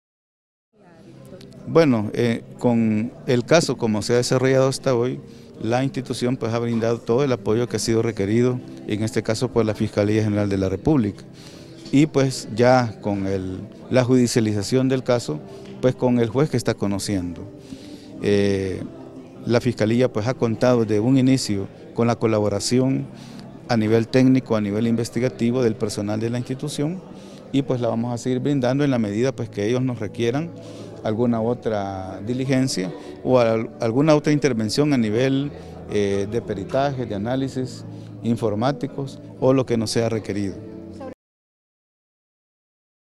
N11 Reacciones de Director de la PNC en caso troll center
El director de la policía afirmó que colaboraran con la fiscalía para ampliar la investigación en el caso de la empresa bunker  o troll center, desde la cual se habrían realizado fraudes cibernéticos